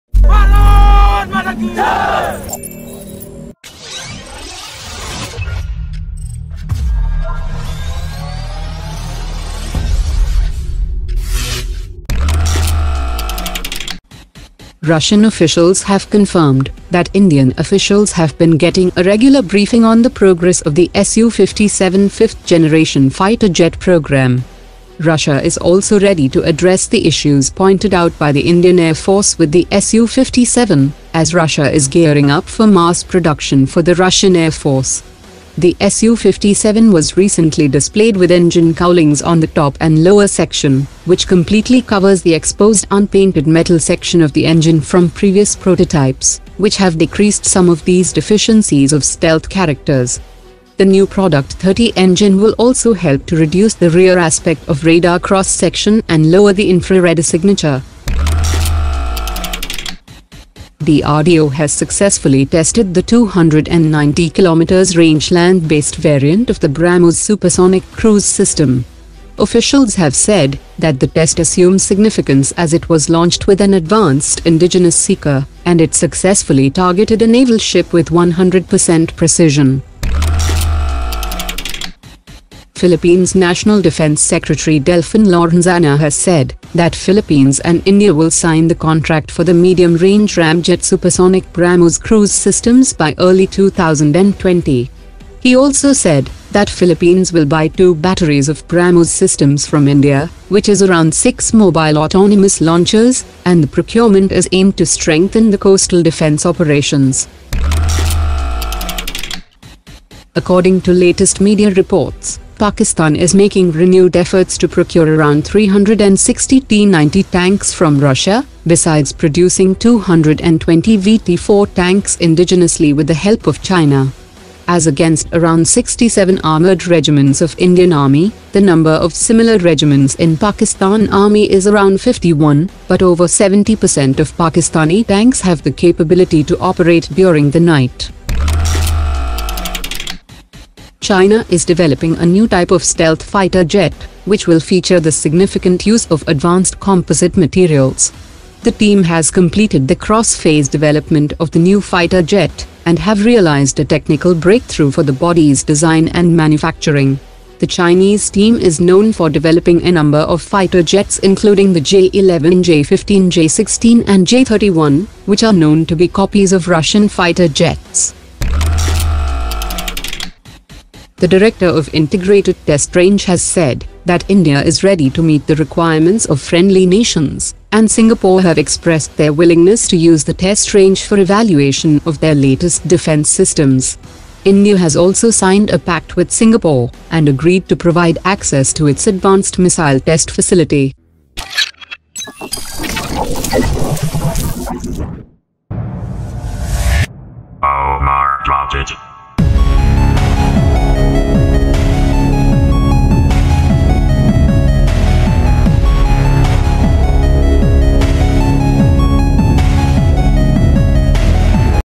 Indian Defence Updates : New BrahMos Seeker,Su-57 Stealth Boost,360 T-90 To PAK,BrahMos Deal 2020
1. India being briefed regularly on Su57EIndia Program By Russia.
2. DRDO Tests 290 Km range Land Based BrahMos with New Advanced Indigenous Seeker.
3. Philippines Defence Secretary confirms BrahMos Deal By Early 2020 : 2 Batteries
4. Pakistan Buying 600 Tanks : 360 T-90 from Russia,240 VT-4 Chinese Tanks.
5. India Offers Integrated Test Range to friendly nations.
6. China begins development/copying NewStealthFighter Jet.